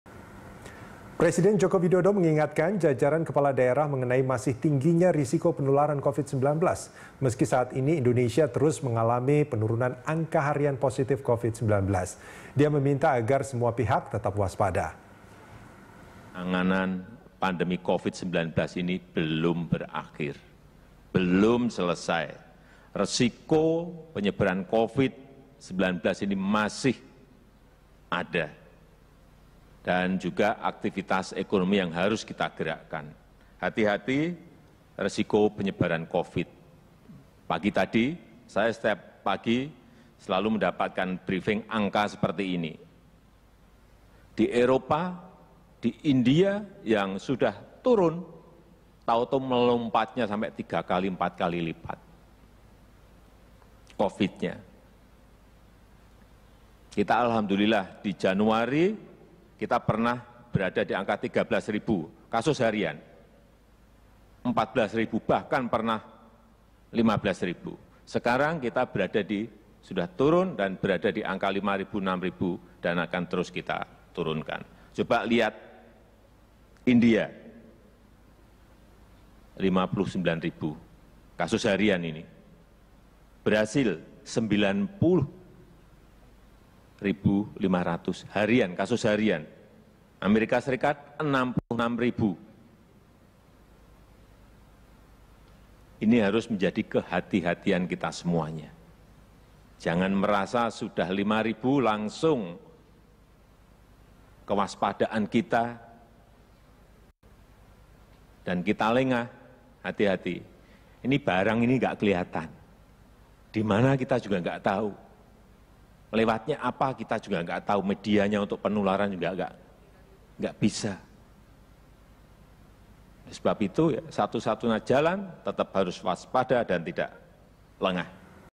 Jokowi Ingatkan Seluruh Kepala Daerah Waspadai Covid-19
Presiden Joko Widodo (Jokowi) ingatkan jajaran kepala daerah mengenai masih tingginya risiko penularan virus corona atau Covid-19. Meski saat ini Indonesia terus mengalami penurunan angka harian positif Covid-19, dia meminta agar semua pihak tetap waspada.